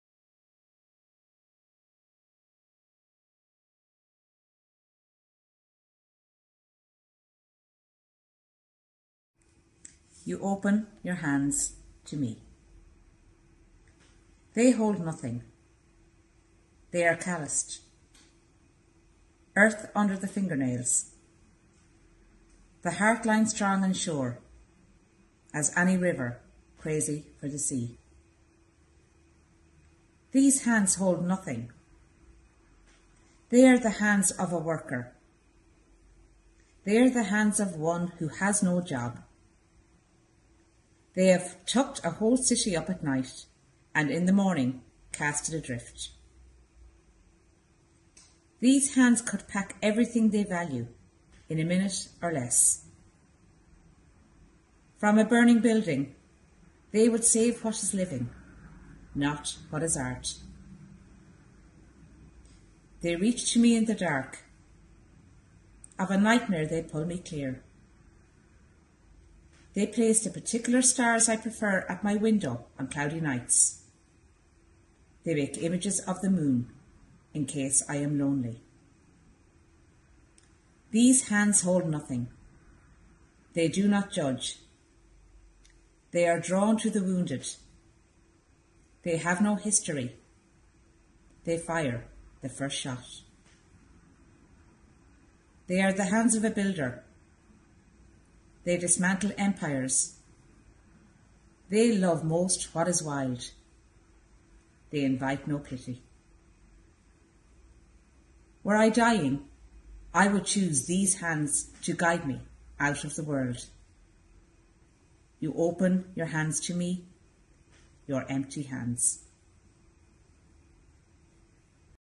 Bealtaine Poetry Sessions at 11: "You Open your Hands to me"
Our colleague Genevieve Montgomery, Ballymahon Community Library Staff Member, reads "You Open your Hands to me", by Paula Meehan, from  the collection "As If By Magic".